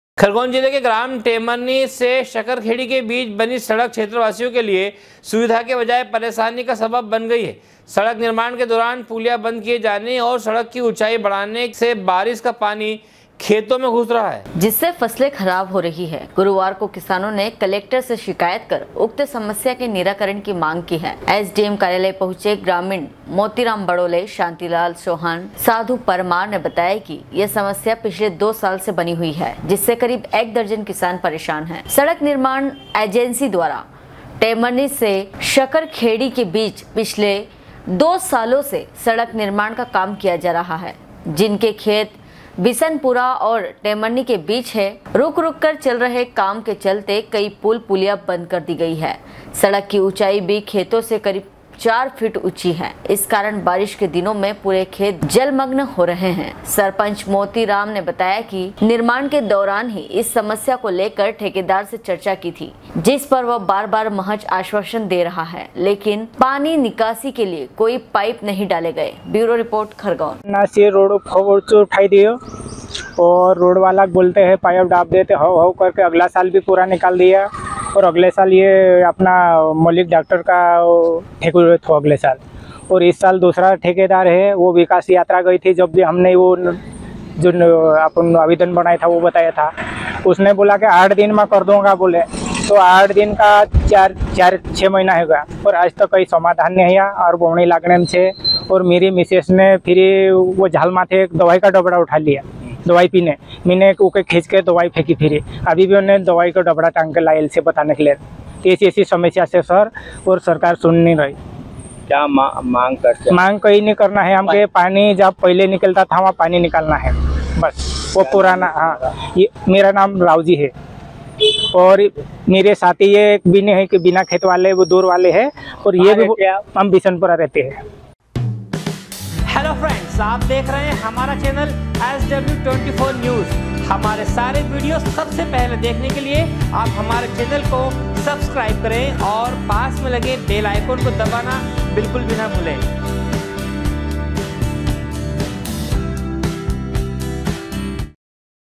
ग्राम टेमरनी से शकरखेड़ी के बीच बनी सड़क क्षेत्रवासियों के लिए सुविधा के बजाय परेशानी का सबब बन गई है।
खरगोन जिले के ग्राम टेमरनी से शकरखेड़ी के बीच बनी सड़क क्षेत्रवासियों के लिए सुविधा के बजाय परेशानी का सबब बन गई है। सड़क निर्माण के दौरान पुलिया बंद किए जाने से और सड़क की ऊंचाई बढऩे से बारिश का पानी खेतों में घुस रहा है